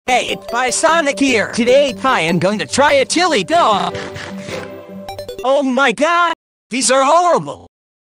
sonic tries a chili dog
for the t4thdoh50k thing.
made this on dreams
sonic voice from vo.codes